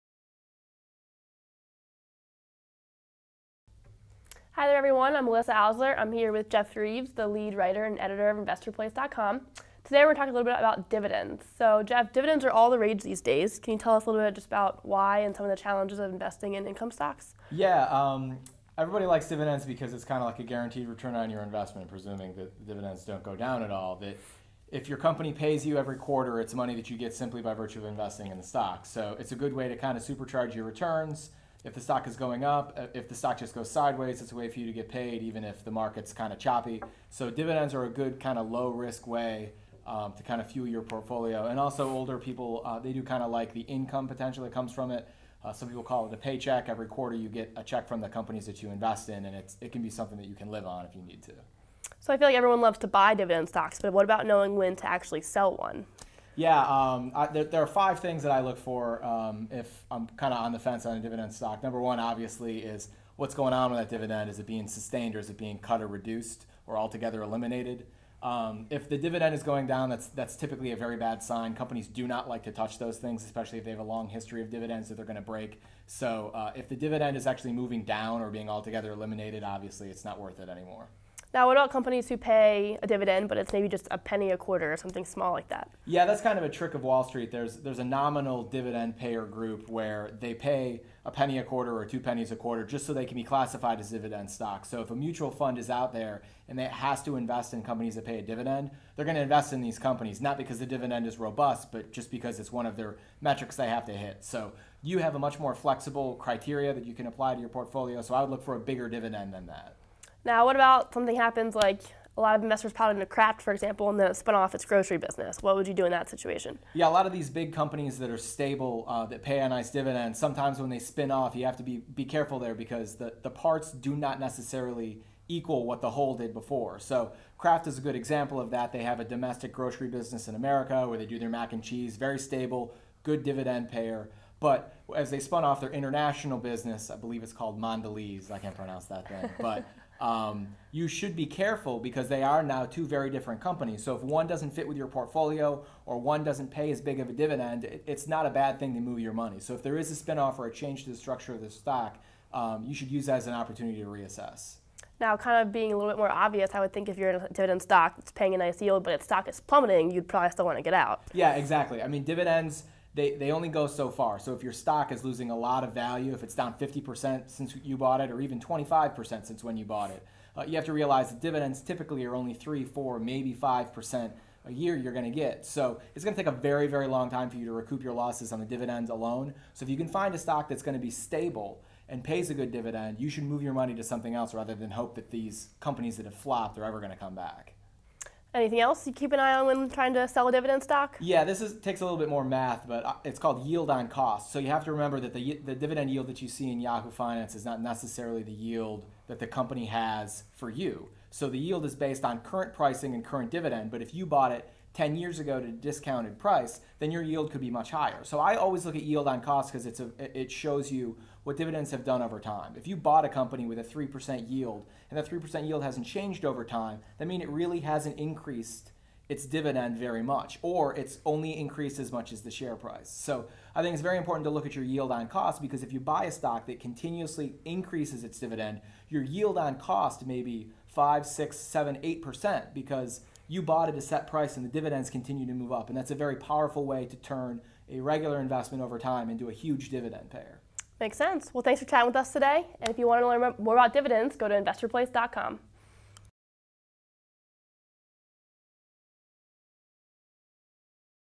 When Should You Sell Your Dividend Stocks?
Jeff Reeves, editor of InvestorPlace.com, talks with assistant editor Alyssa Oursler about when it may be time for you to sell your dividends stocks